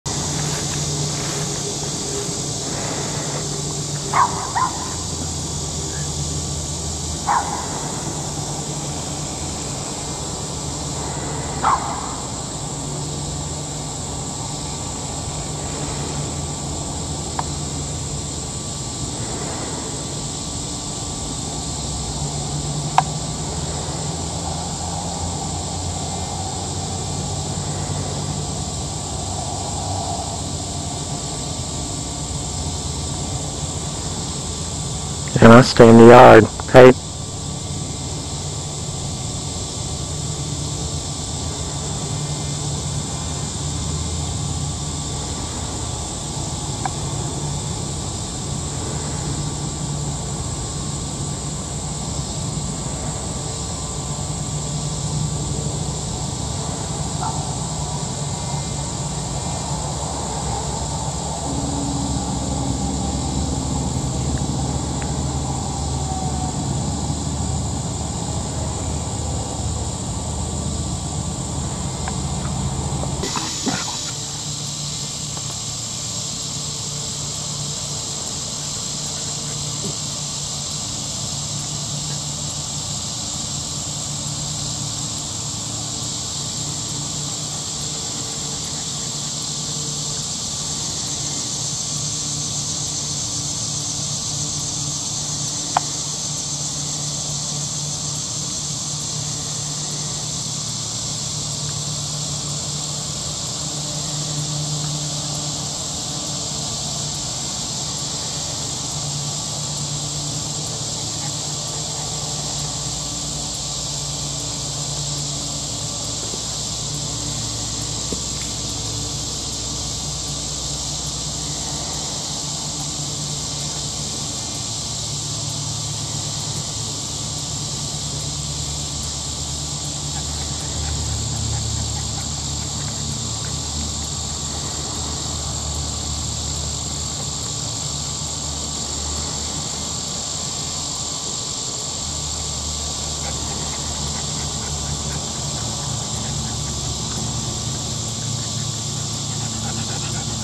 Schnauzer And The Rabbit
Our seven year old schnauzer, Ema, is tempted to leave the yard to chase rabbits.